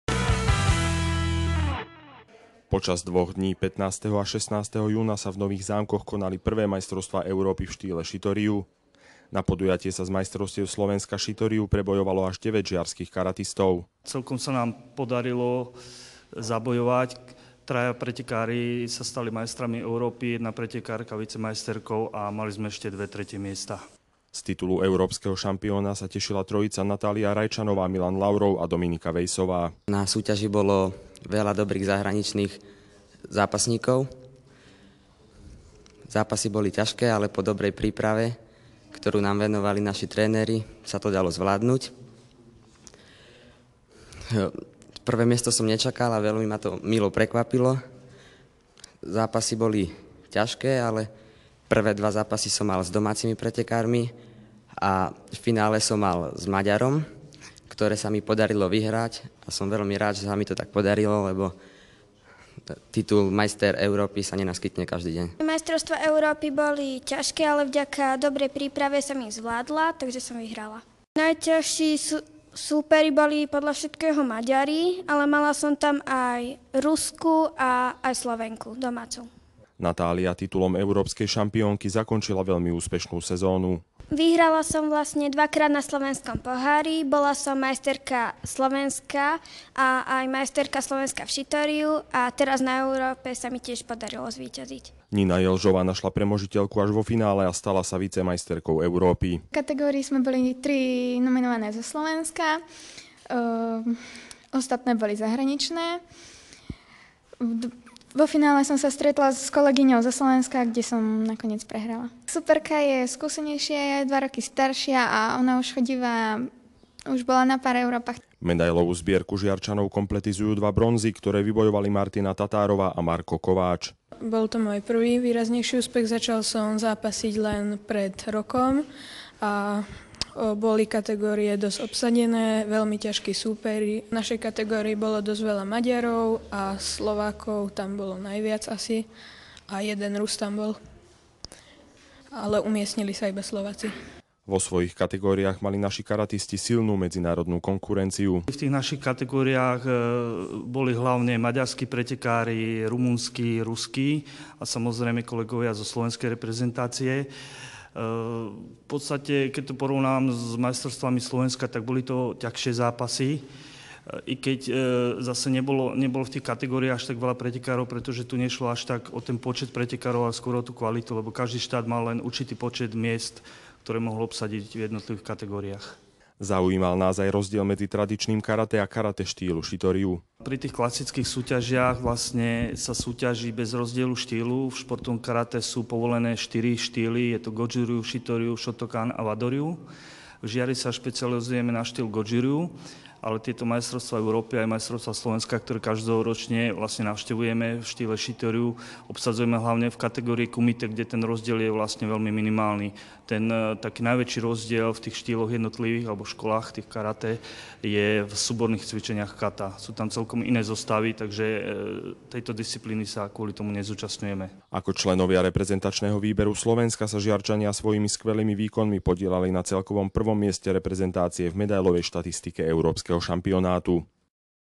Žiar má troch majstrov Európy
Karate svojimi výsledkami patrí medzi najúspešnejšie športy v Žiari nad Hronom. Karate klub MŠK každoročne vychováva majstrov republiky a medailistov zo svetových a európskych pohárov.